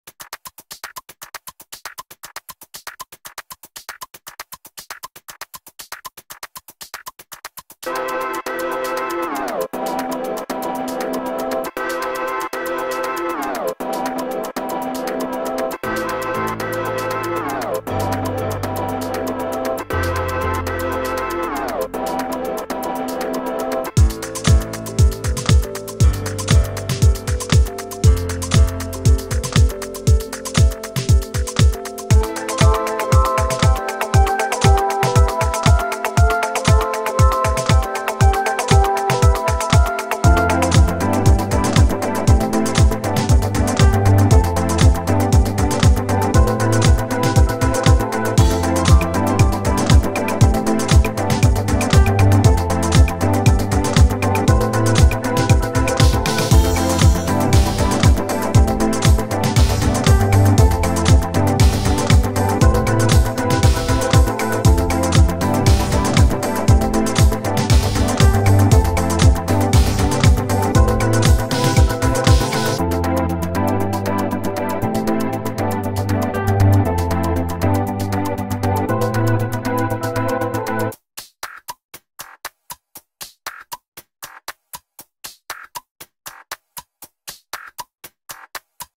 Animation Speedpaint * Garageband music
Animated/drawn on: Procreate (15 frames, 10 fps)
Edited on: VLLO
Music made on: GarageBand